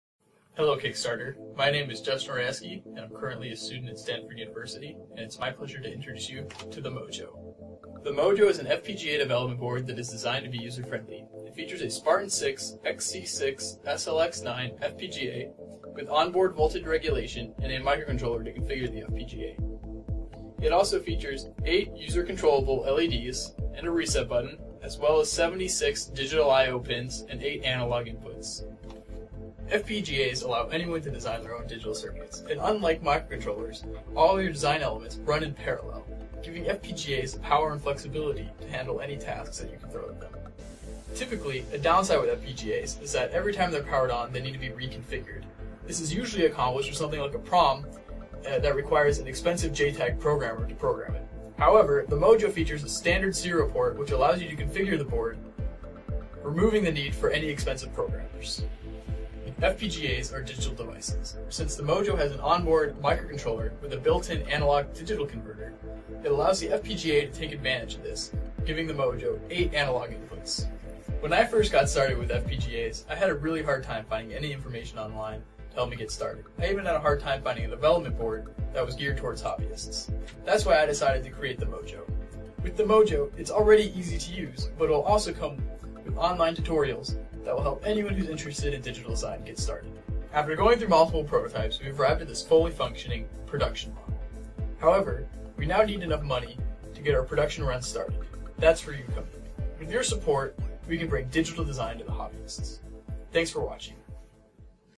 Mojo Digital Design for the Hobbyist
The Mojo is an FPGA development board that is designed to be user friendly and a great introduction into digital design for anyone.